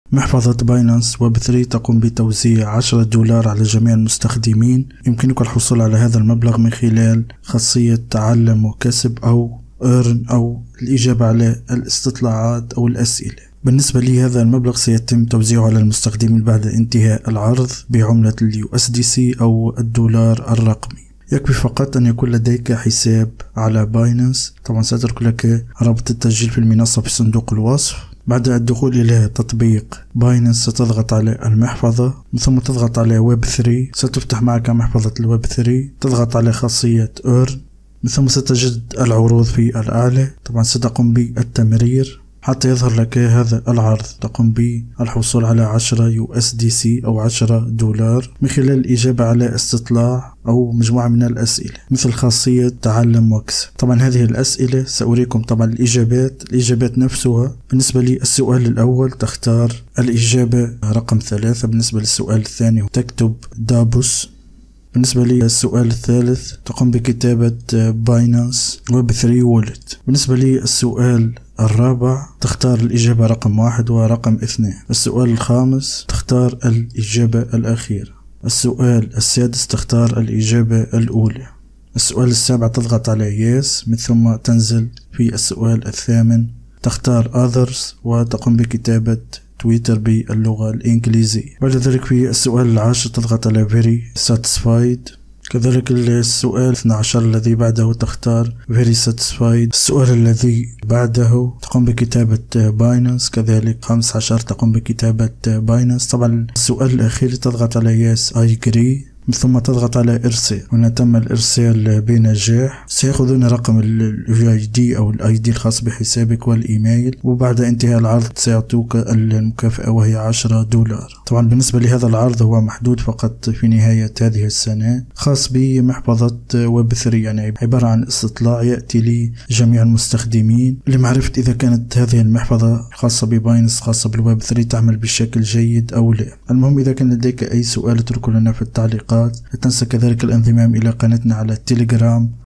توزيع 10$ من محفظة بينانس ويب 3 على جميع المستخدمين
بمناسبة نهاية السنة تقوم محفظة بينانس ويب 3 بتوزيع 10$ على جميع المستخدمين من خلال خاصية تعلم واكسب.
رابط التسجيل إفتحه من الهاتف👇
قناتنا على التيليجرام👇
▪ أفضل البنوك و المحافظ الإلكترونية👇
توزيع 10$ من محفظة بينانس 💸 | تعلم وكسب في ويب 3 | جميع الاجوبة الصحيحة للمشاركة في المسابقة